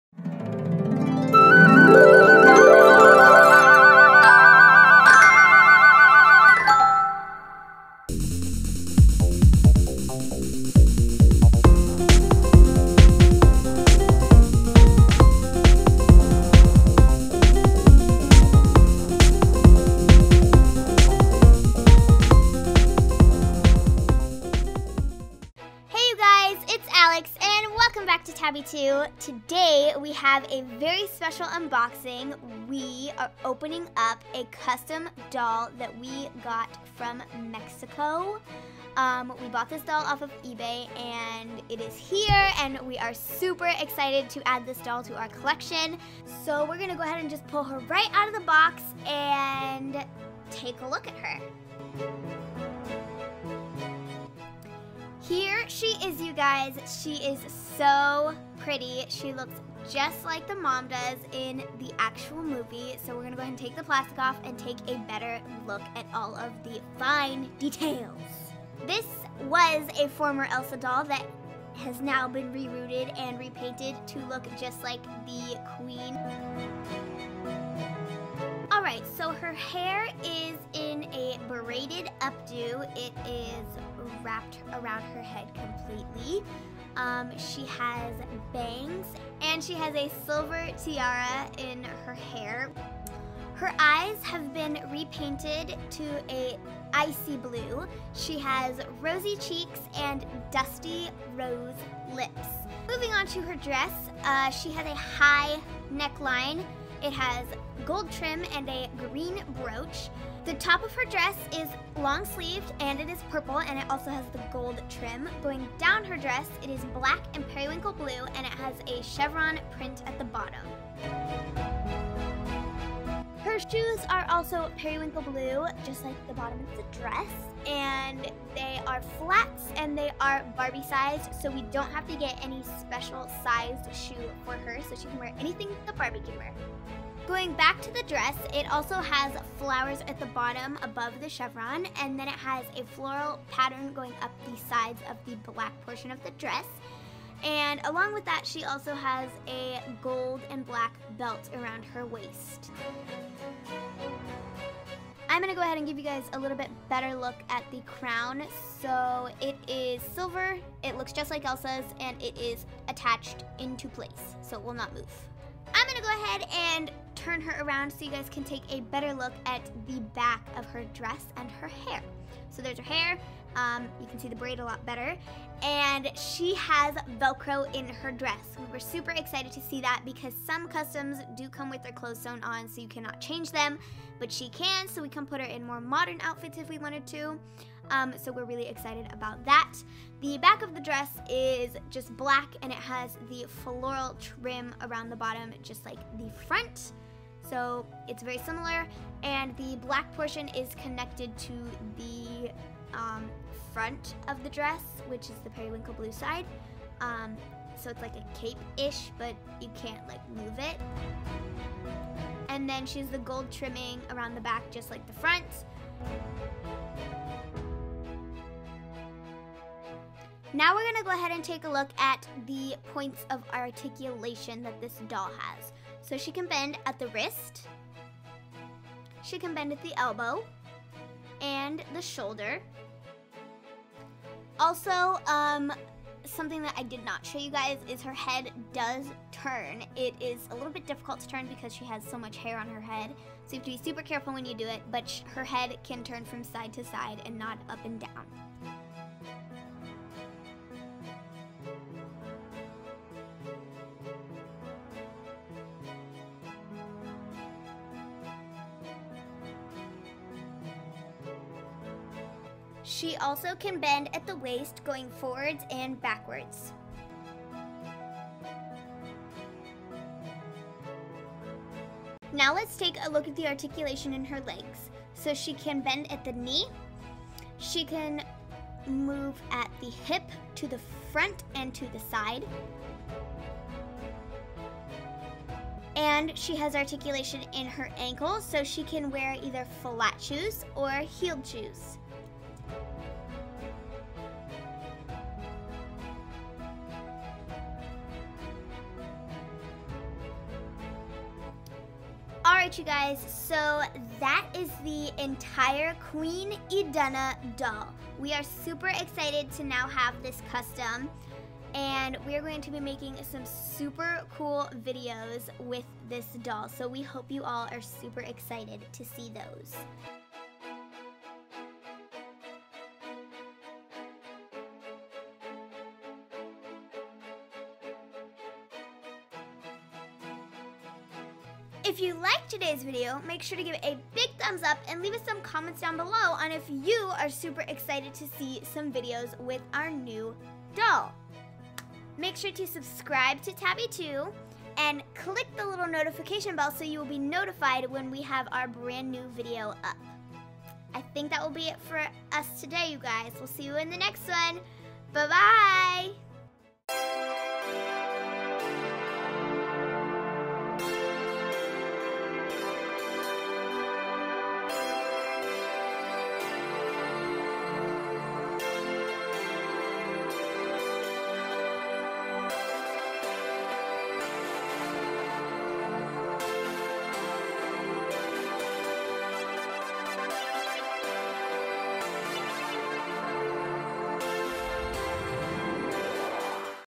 AMAZING Disney FROZEN Elsa Anna's MOM Queen IDUNA Custom OOAK Collector Parents Barbie Doll Unboxing
We have a very special doll video today...unboxing our first custom Barbie, QUEEN IDUNA of Arendelle, Elsa & Anna's MOM! We are proud to add her to our ever-growing doll collection and super excited to make videos using this doll!